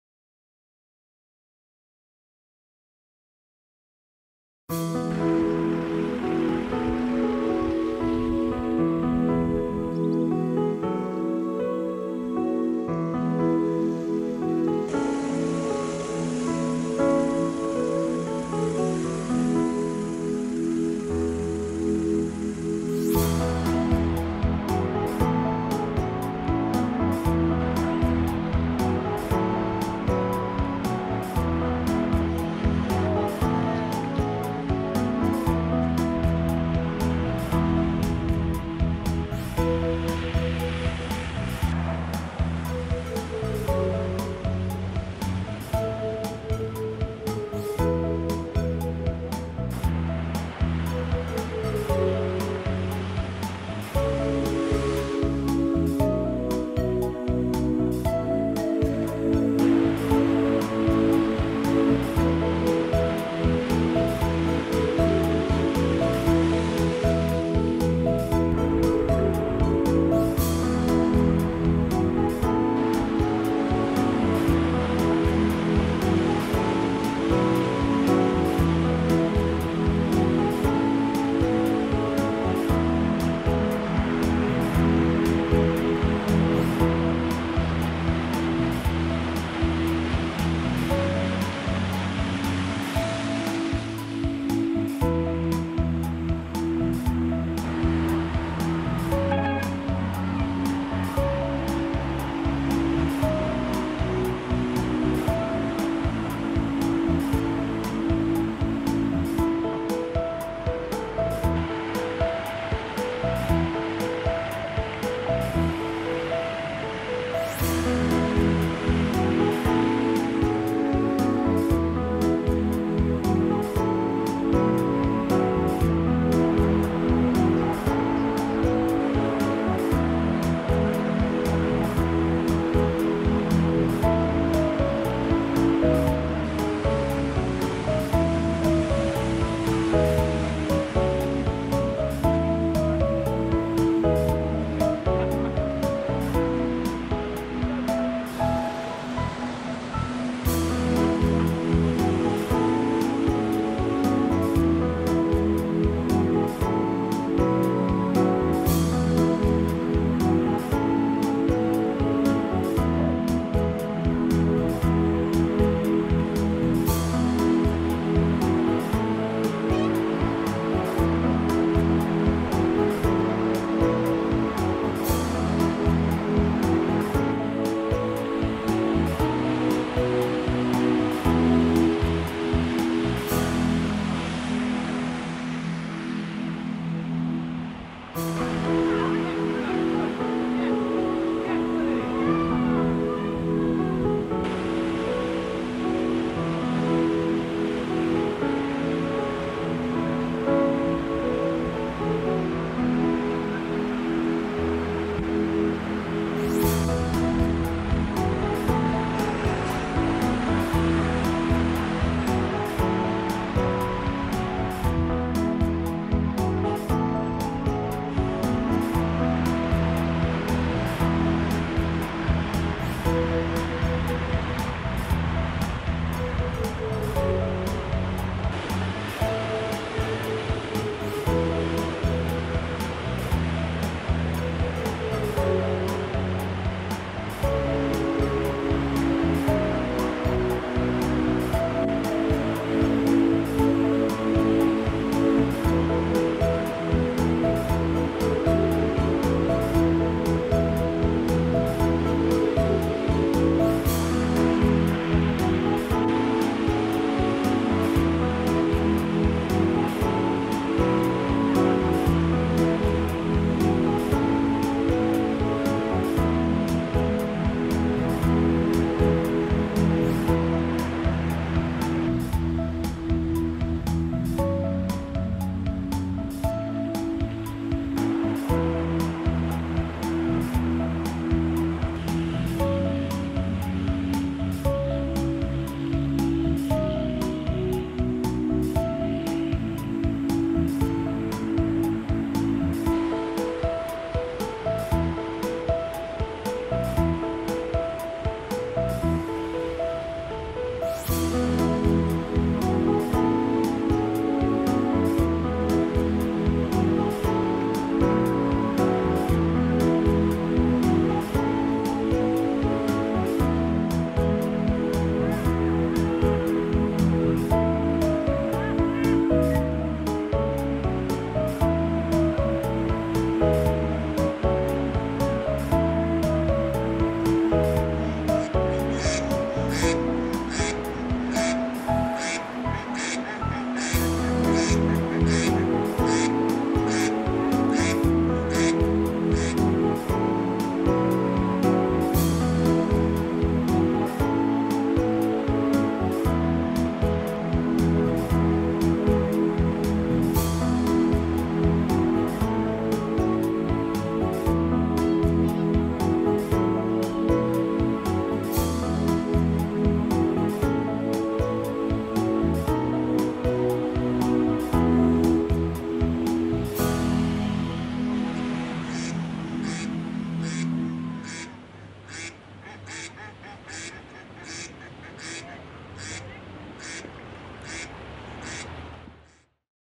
Kenilworth Ford Floods 2018 - Van Gets Stuck. Warwickshire, England
Another "Flash" flood at the Ford and Kenilworth is in chaos again. Most drivers took the diversion routes but plenty went through. Not every vehicle made it though!